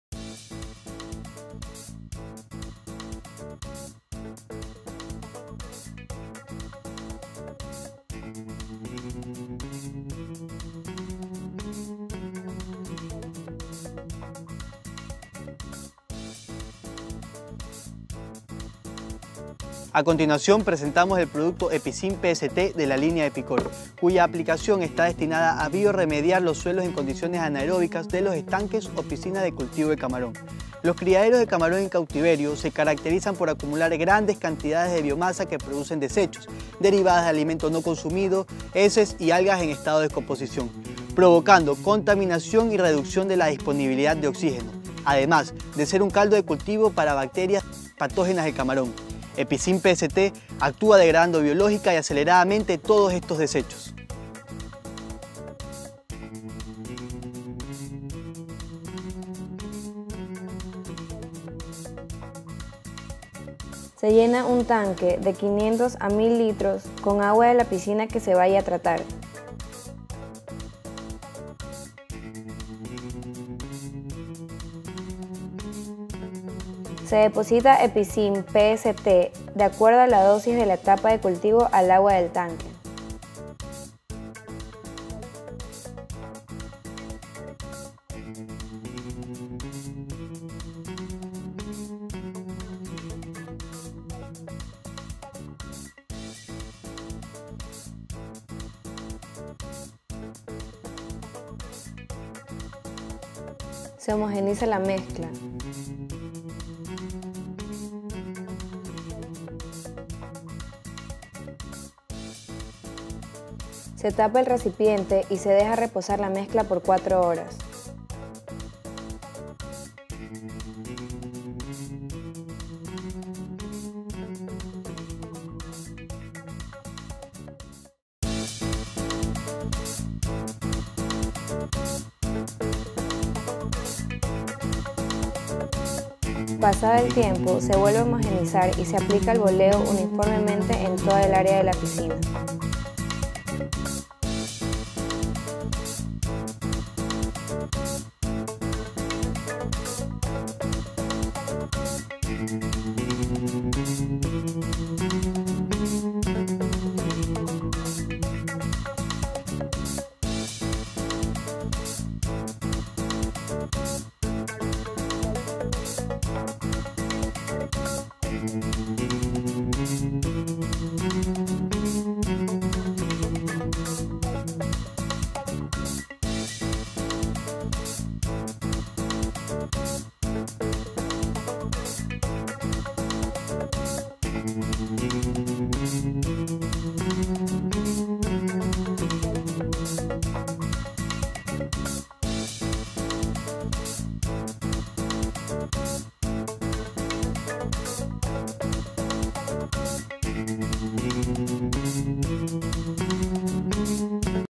EPICIN PST [EPIZYM PST] video español subtitled in English
Como usar el product EPIZYM PST (EPICIN PST), Bioremediador de suelos de piscinas y tanques de cultivo acuícola. MEGASUPPLY.